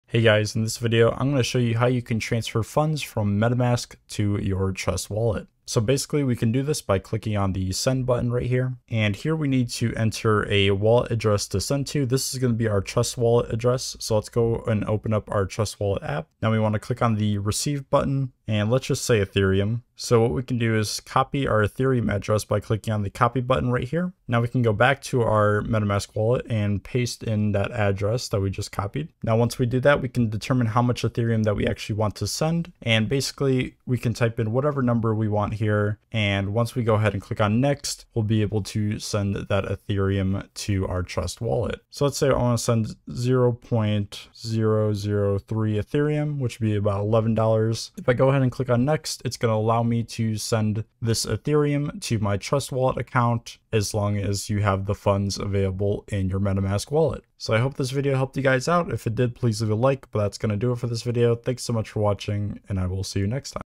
How to Transfer Crypto from Metamask to Trustwallet (2022)
In this video, we go over how you can transfer funds from Metamask to Trustwallet. To transfer crypto Bitcoin from Metamask to Trustwallet, you may need to look into WBTC, which is a an ERC-20 token that represents Bitcoin on the Ethereum blockchain.